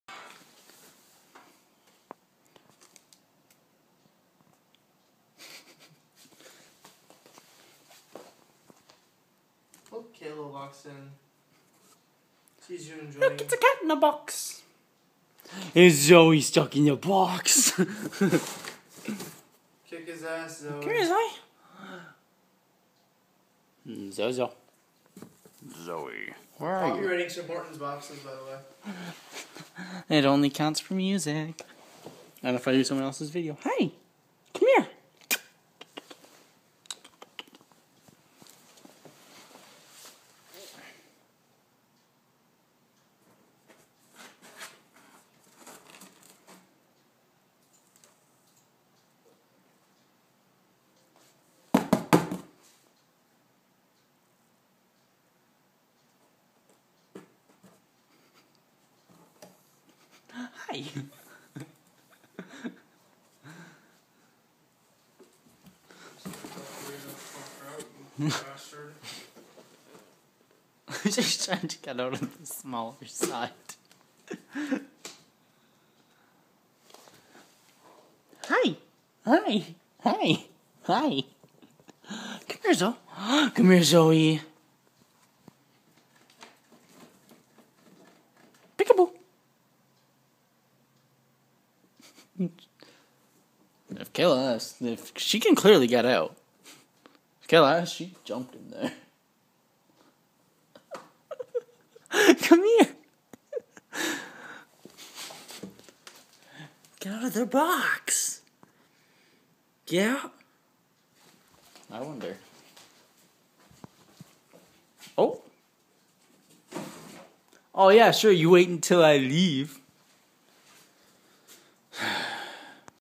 cute cat stuck in a box
my cat zoe stuck in a box haha